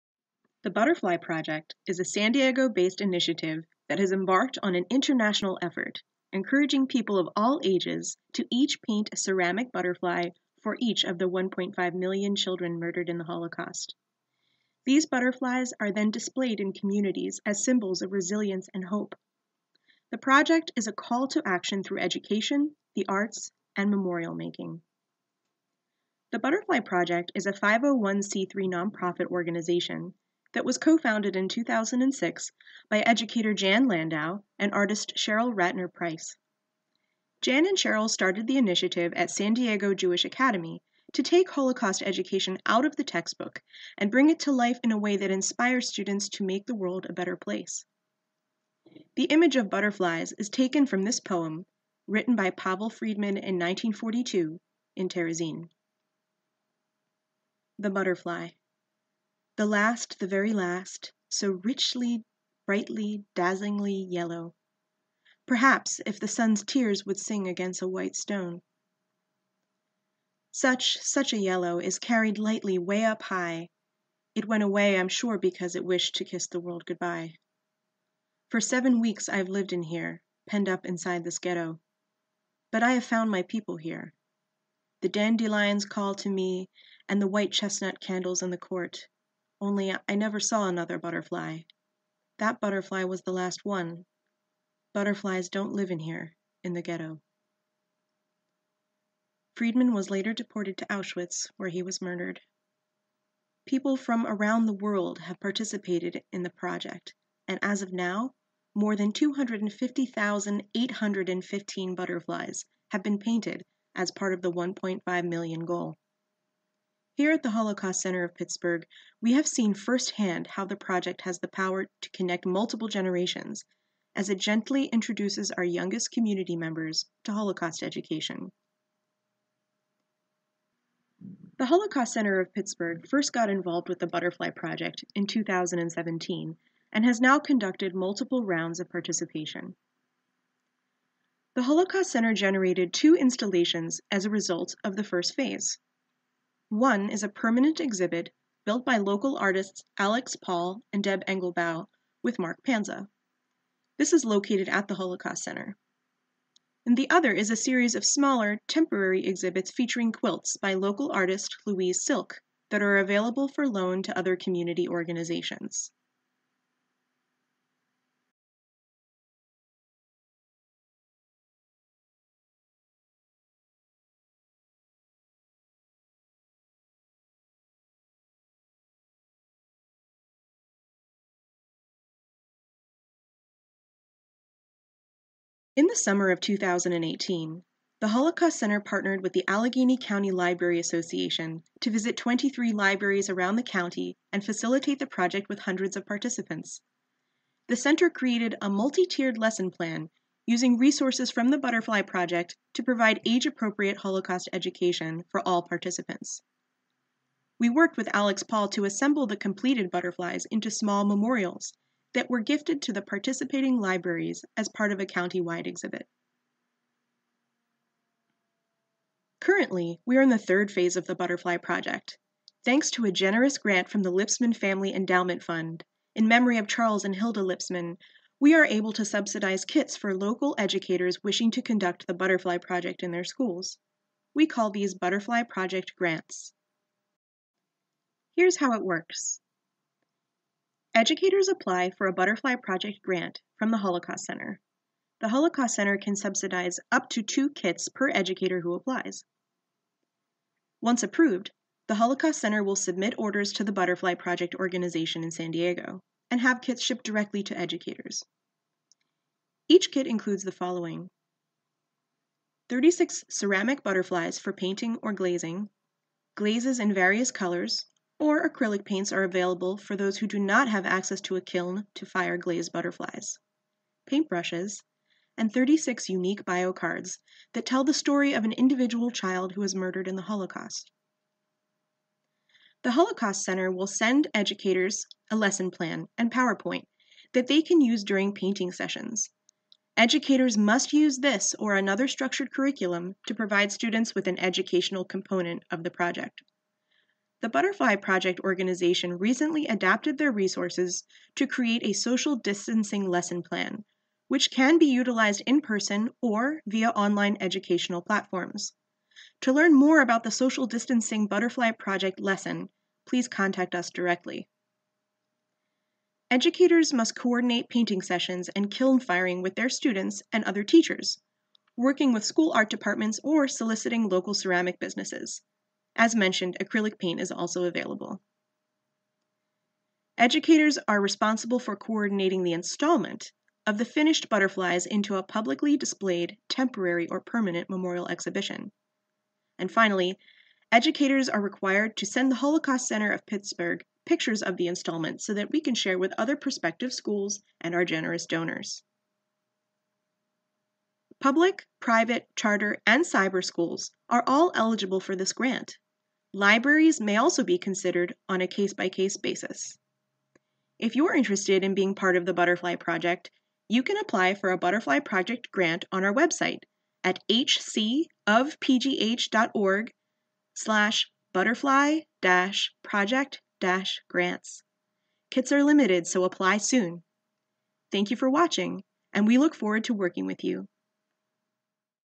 Butterfly Project Webinar
Short informational video on the history of the Holocaust Center's involvement with the Butterfly Project and how teachers can bring the project to their own schools. Thanks to our generous sponsors, funding is available from the Holocaust Center of Pittsburgh on a limited basis.